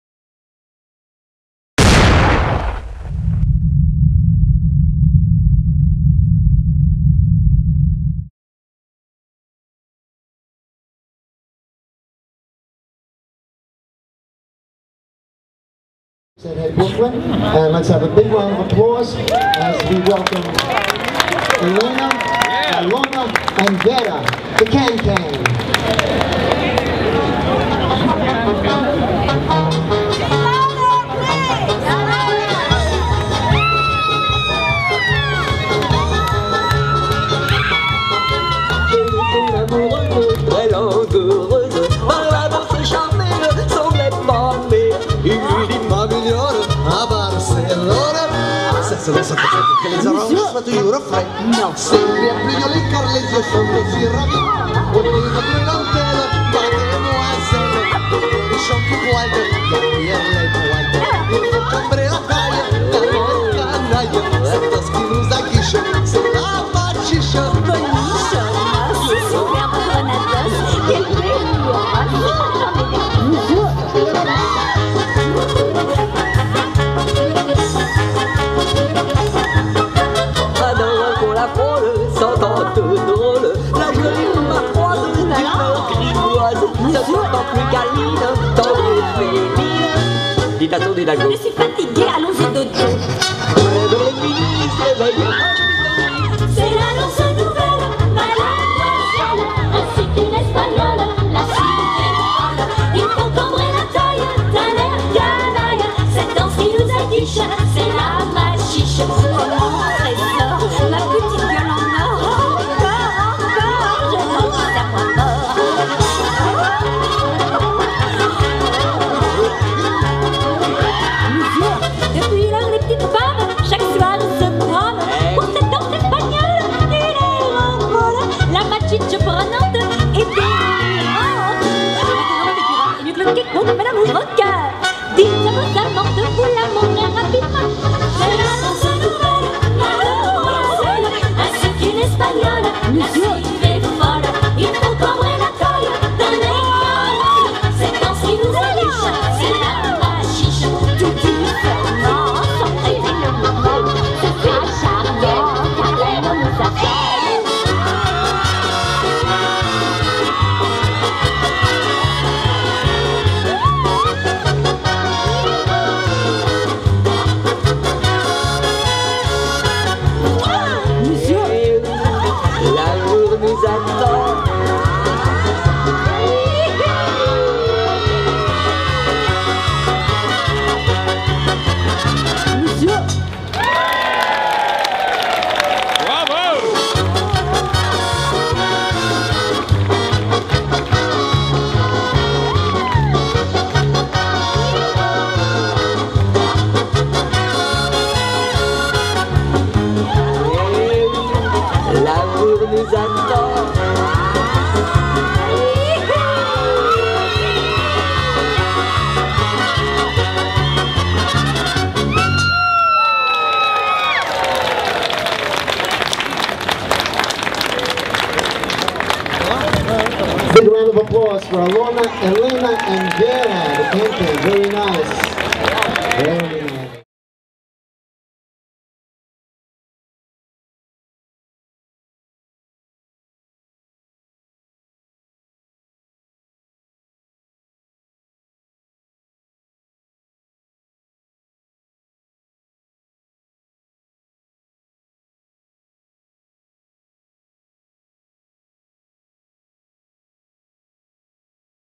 Bastille Day Cancan - July 2009
Live from NYC's French Institute/Alliance Francaise Bastille Day street fair!  Three dancers from Rasputin's Follies in Brooklyn kick up their heels for the entertainment of all and sundry.  This clip was knitted together from three separate performances.  (And if you like the dancers as much as I did, they're are available for hire through Barynya Entertainment: